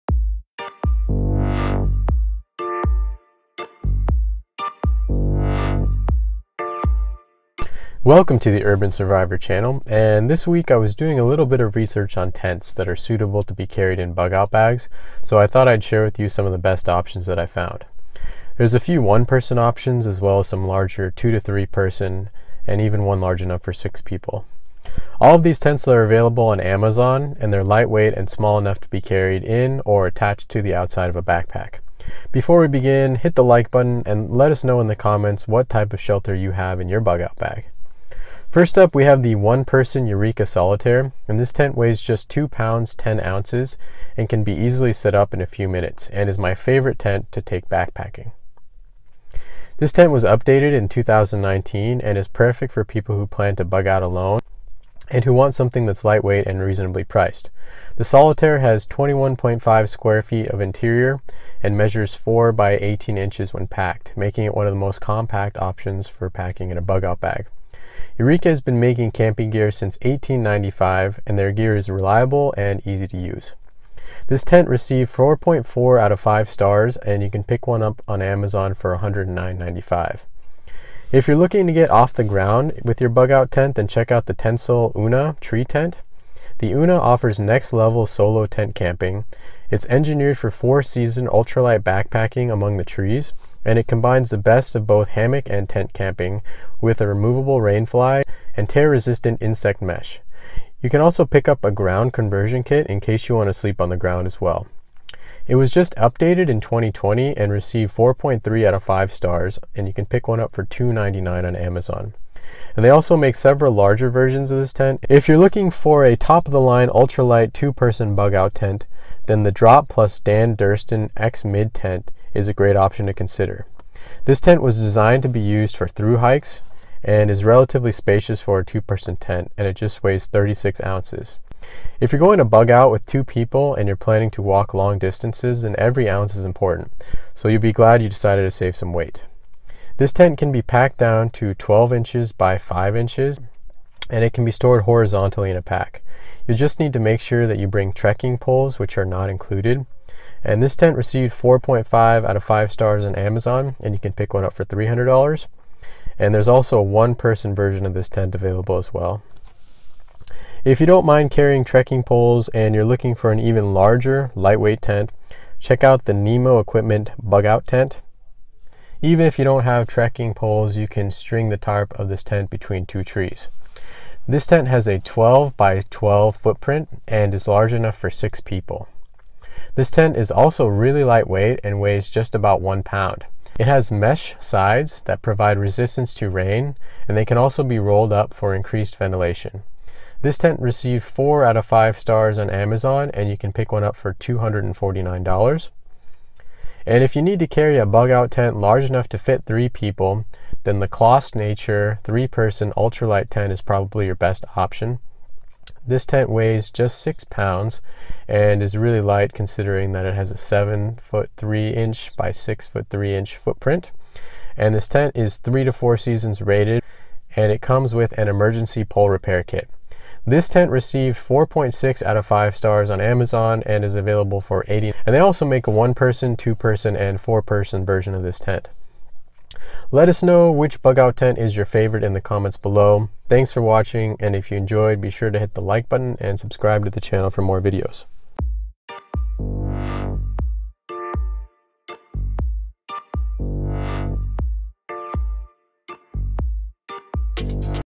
The 5 Best Bug Out Bag Tents
Welcome to the Urban Survivor channel and this week I was doing a little bit of research on tents that are suitable to be carried in bug out bags so I thought I’d share with you a few of the best 5 Bug Out Bag Tents I found!

Eureka Solitaire AL 1 Person 3 Season - $109.95
Tentsile UNA Tree Tent - $299
Drop + Dan Durston 2 Person Ultralight Tent - $300
Nemo Equipment Bug Out Tent - $249.95
Clostnature 3 Person Ultralight Tent - $89.99

There’s a few 1 person options as well as some larger 2-3 person and even one large enough for 6 people.

First up we have the 1 Person Eureka Solitaire. This tent weighs just 2 pounds 10 ounces and can be easily set up in a few minutes and is my personal favorite tent to take backpacking. This tent was updated in 2019 and is perfect for people who plan to bug out alone and want something that’s lightweight and reasonably priced. The Solitair has a 21. 5-square-foot interior and measures 4 by 18 inches when packed making it one of the most compact options for packing in a bug out bag. Eureka has been making camping gear since 1895 and their gear is all reliable and easy to use. This tent received 4.4 out of 5 stars and you can pick one up on Amazon for $109.95.

If you are looking to get off the ground with your bug out tent then check out the Tentsile UNA tree tent. The UNA offers Next-level solo tent camping. It’s engineered for 4 season ultralight backpacking among the trees. It combines the best of both hammock and tent camping, with removable rainfly and  tear-resistant insect mesh. You can also pick up a ground conversion kit in case you want to sleep on the ground as well. It was just updated in 2020 and received 4.3 out of 5 stars and you can pick one up for $299 on Amazon. They also make several larger versions of this tent in case you need a bug out shelter for more people.

If you are looking for a top of the line ultralight 2 person bug out tent, the Drop + Dan Durston X-Mid Tent is a great option to consider. This tent was designed to be used for thru hikesThis is relatively spacious for a 2 person tent and it weighs just 36 ounces. If you are going to bug out with 2 people and you’re planning to walk long distances, every ounce is important so you’ll be glad you decided to save some weight. This tent can be packed down to a 12” by 5” pack that can be stored horizontally in a pack. You’ll just need to make sure you bring trekking poles which are not included. This tent received 4.5 out of 5 stars on amazon and you can pick one up for $300. There’s also a 1 person version of this tent available as well.

If you don’t mind carrying trekking poles and you are looking for an even larger lightweight tent, check out the Nemo Equipment bug out tent.  If you don’t have trekking toles you can also string the tarp of this tent between 2 trees. This tent has a 12 by 12 footprint and is large enough for six people. This tent is also really light and weighs just about 1 pound. It has mesh sides that provide resistance to rain and they can also be rolled up for increased ventilation. This tent has received 4 out of 5 stars on amazon and you can pick one up for $249.

If you need to carry a bug out tent large enough to fit 3 people then the Clostnature 3 Person ultralight tent is probably your best option. The Clostnature weighs just 6lbs which is really light considering it’s large 7'3'' x 6'3 footprint. This tent is 3-4 seasons rated and it even comes with an emergency pole repair kit. This tent received 4.6 out of 5 stars on amazon and is available for $89.99. They also make a 1 person, 2 person and 4 person version of this tent.

Stay up to date with the latest gear, videos and Giveaways!